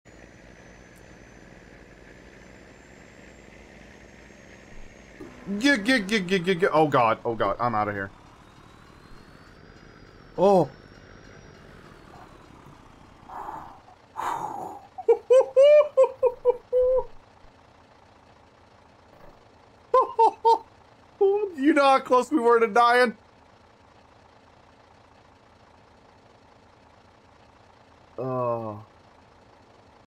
Playing Chicken with a Train
Red trying out his daredevil skills